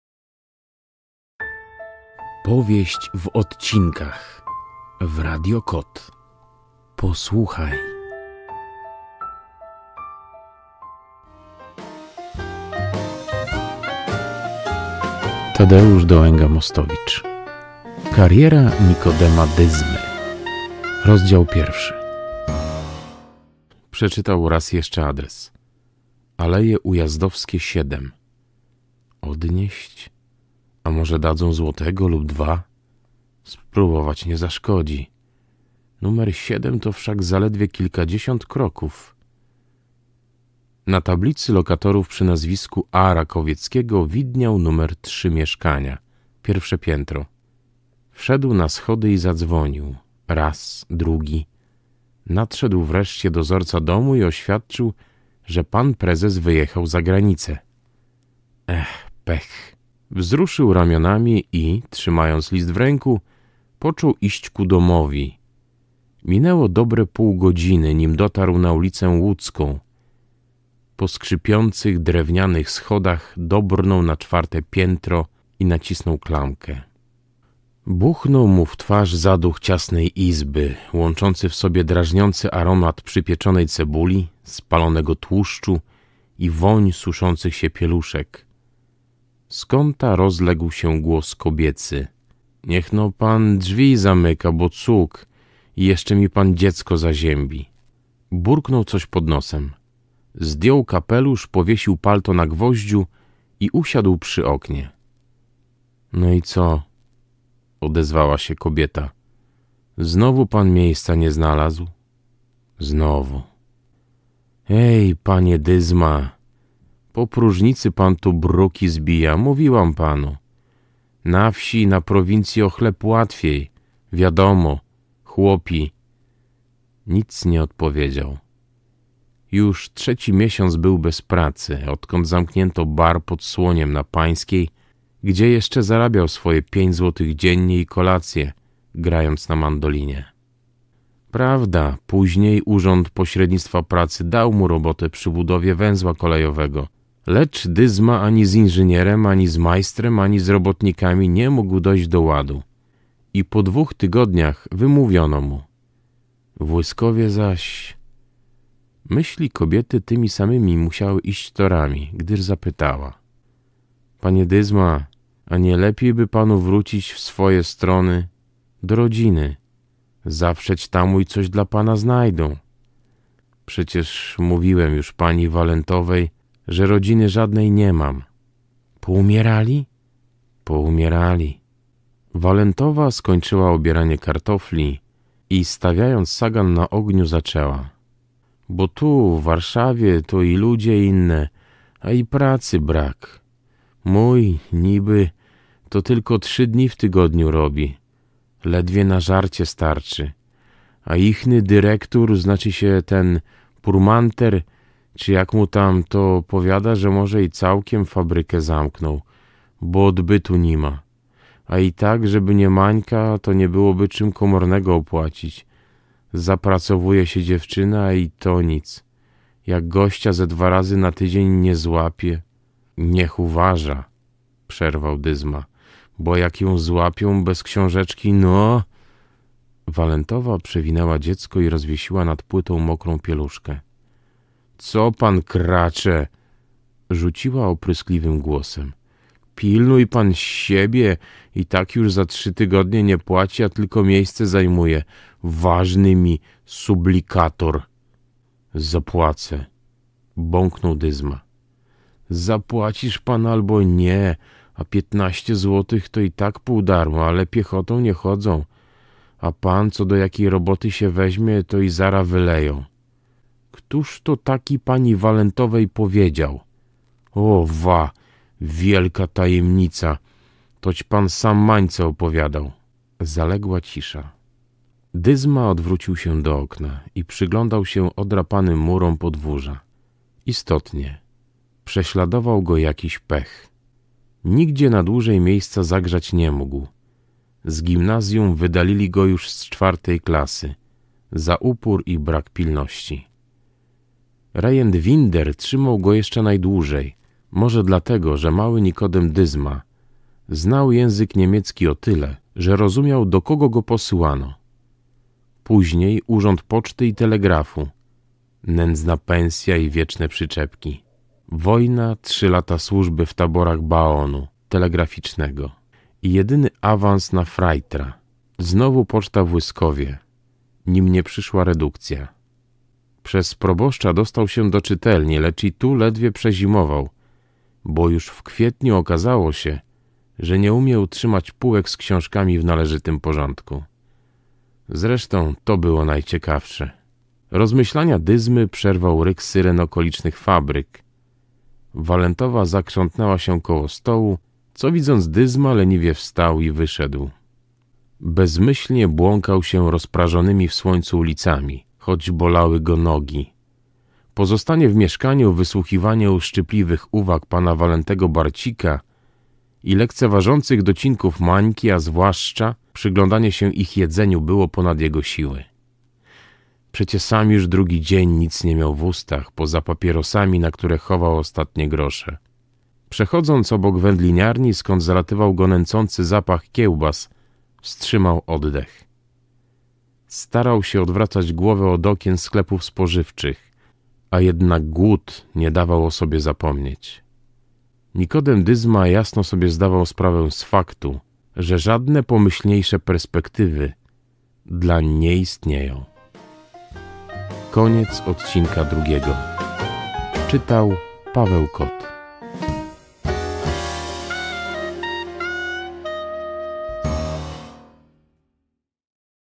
Kariera Nikodema Dyzmy odc. 2- Tadeusz Dołęga-Mostowicz
Powieść w odcinkach w Radio KOT: Kariera Nikodema Dyzmy - rozdział pierwszy - odcinek 2

Autor: Tadeusz Dołęga-Mostowicz
Czyta: Paweł Kot